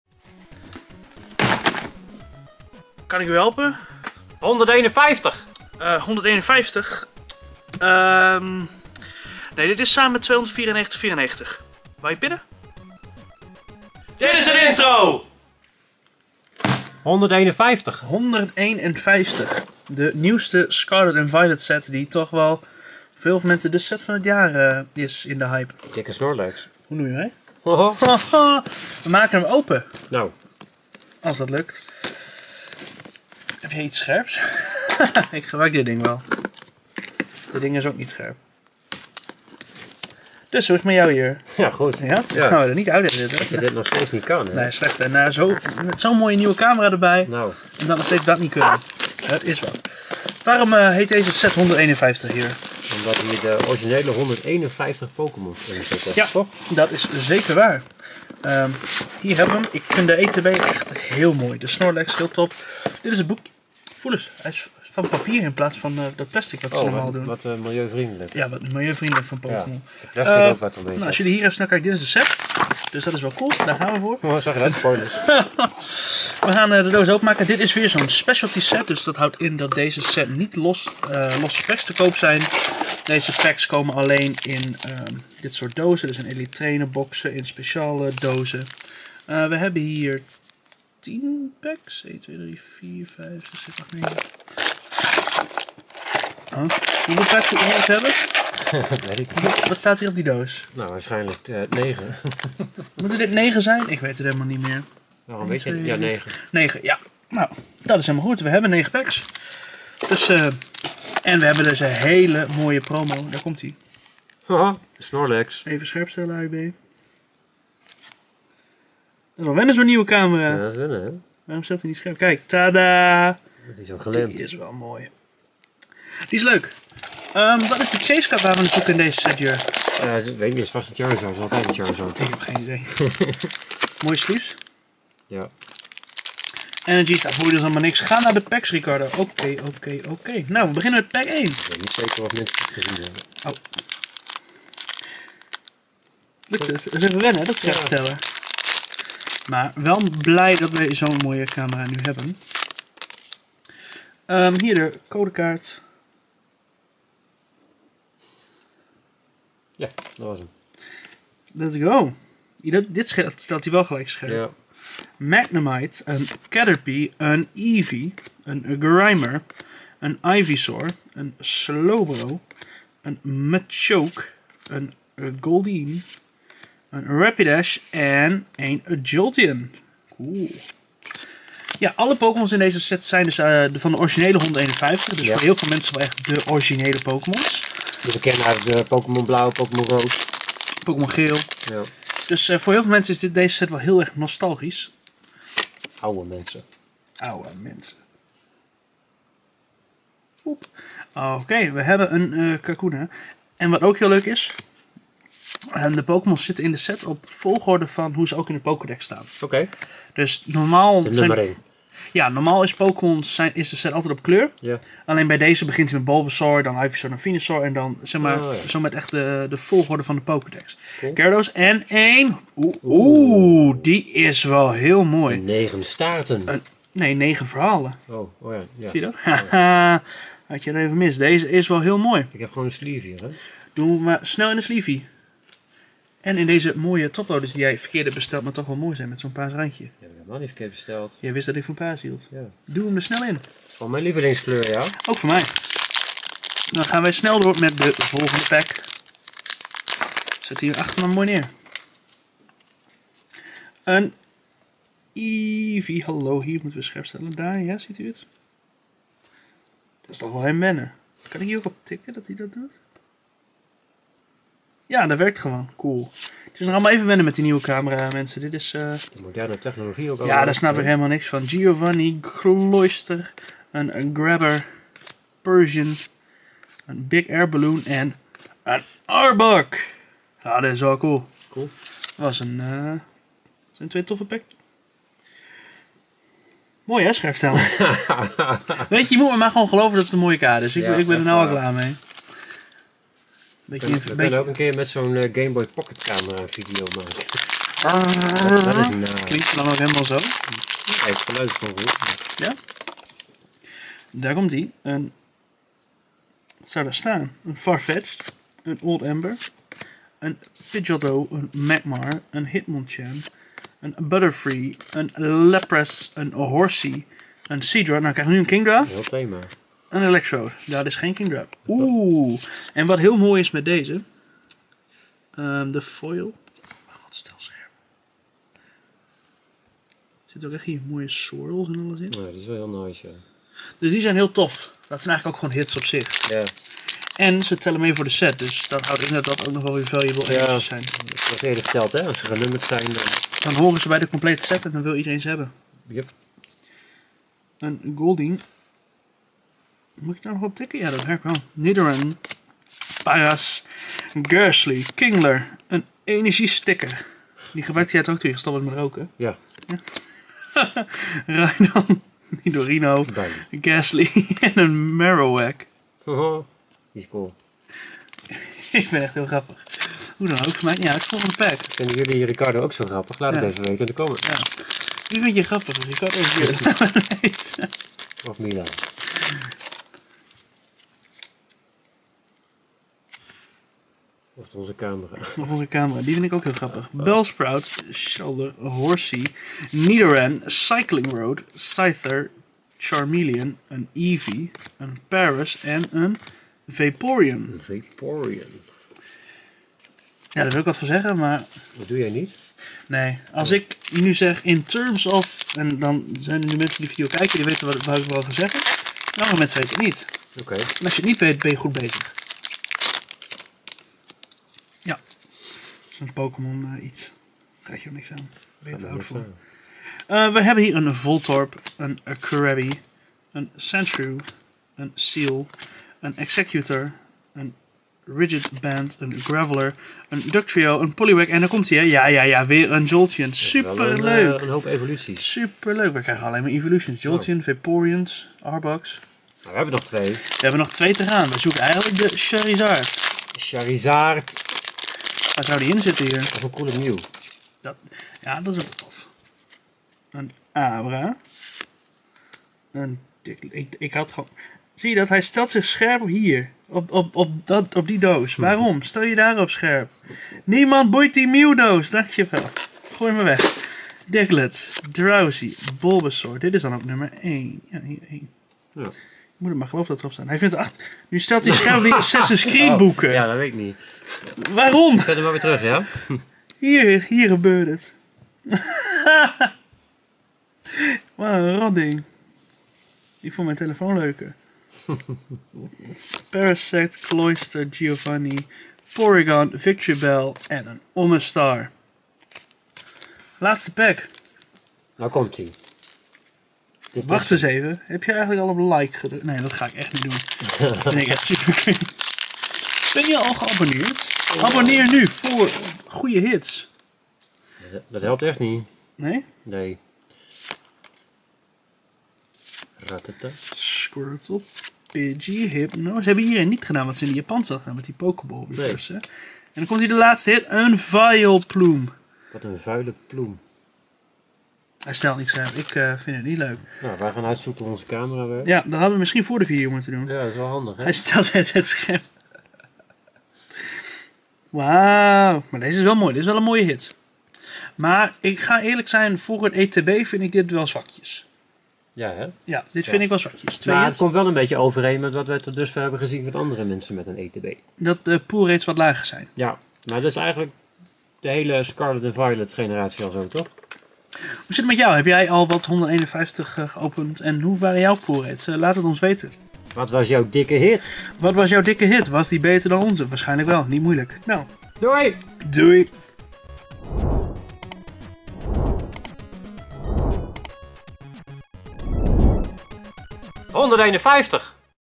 Pokémon Scarlet & Violet 151 Elite Trainer Box
In deze video openen we een Pokémon Scarlet & Violet 151 Elite Trainer Box, hoe tof vinden jullie deze set en wat zijn jullie beste pulls tot dusver?